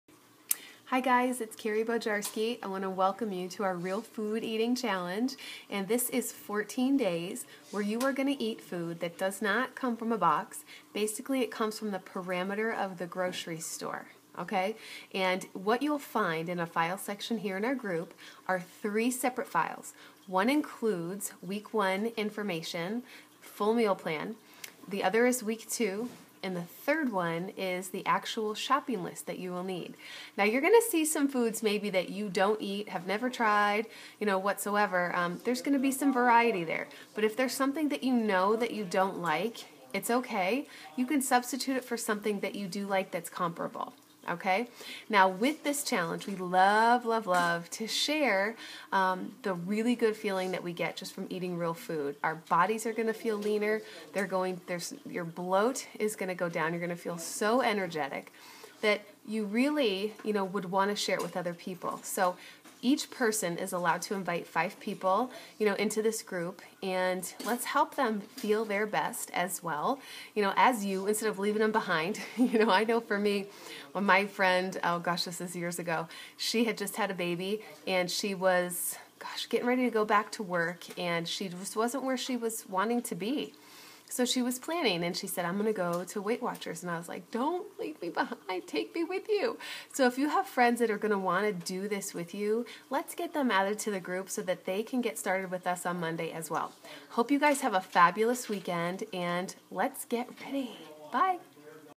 Real food eating Challenge Welcome Video
Do you struggle with FOOD? I offer my REAL FOOD EATING Challenge, monthly. I am giving you a complete menu and a shopping list to take the guess work out of nutrition. I GUARANTEE you will feel better, sleep better and even lose some weight. Average weight loss is 5-12 pounds in 14 days.

Please comment below "I'm in" if you want to join us. again this is absolutely FREE but you are required to check in daily with the group to stay involved-and of course you have to eat REAL FOOD!! READY??

PS- I'm allowing you to invite up to 5 people to join you in the group :)